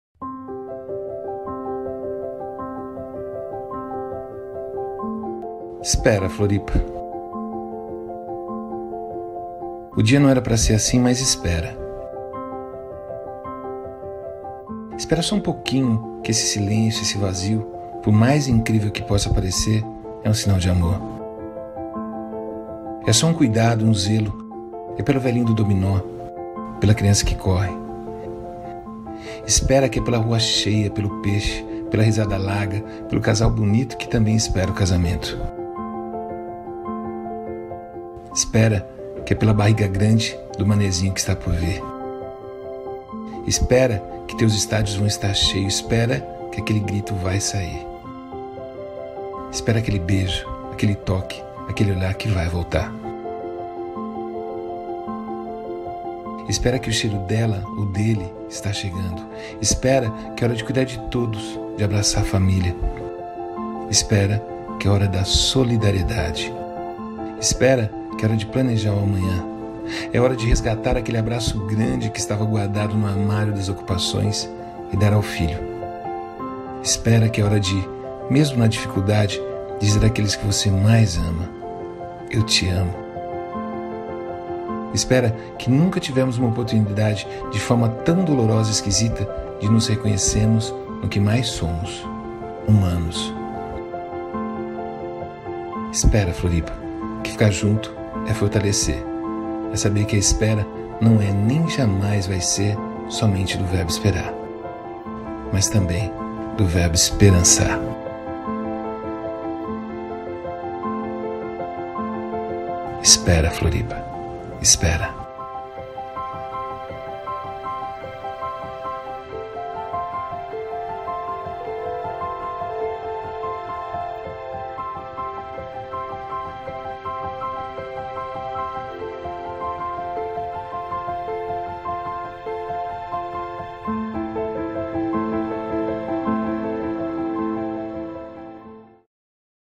Aniversário de Floripa. Silencioso.
Em uma cidade turística como Floripa, este mês de março, foi muito estranho para todos nós habitantes desta linda capital.